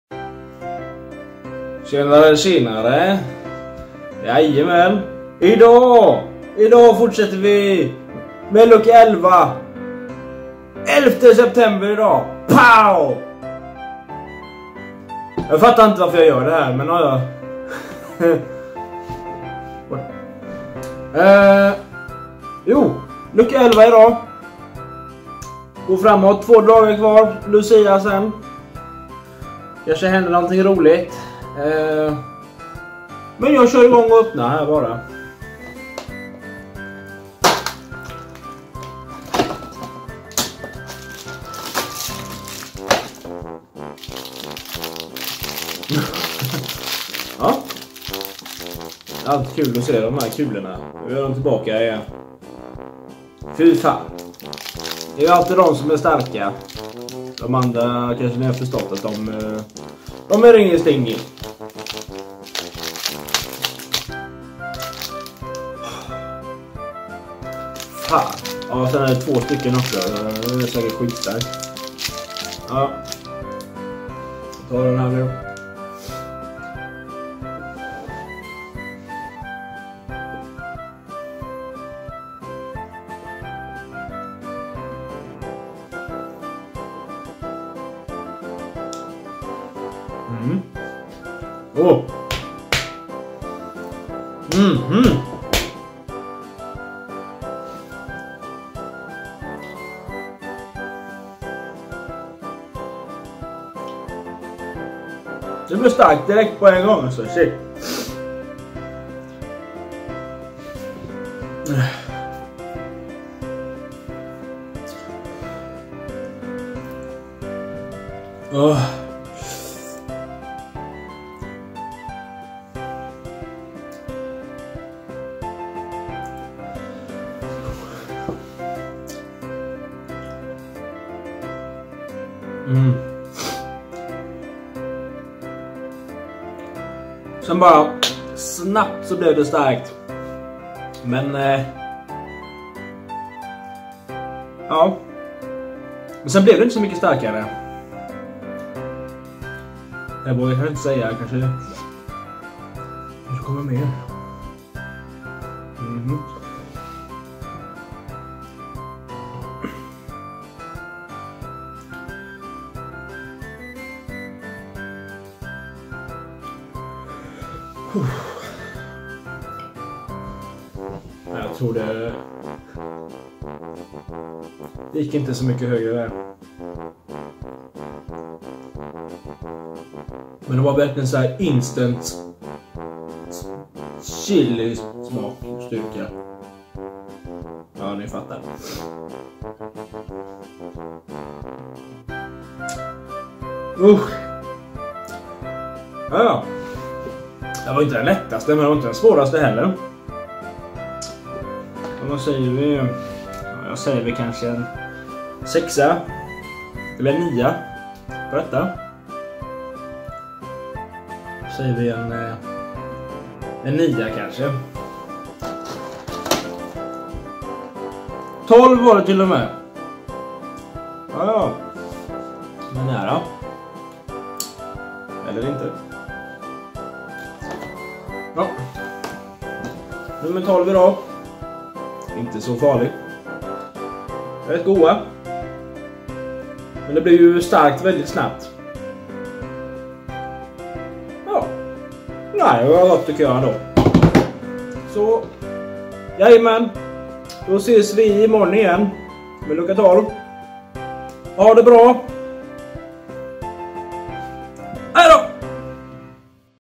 Chili Klaus Julekugler #11 av 25
Jag öppnar och testar Chili Klaus Julekugler och recenserar smaken och chilistyrkan.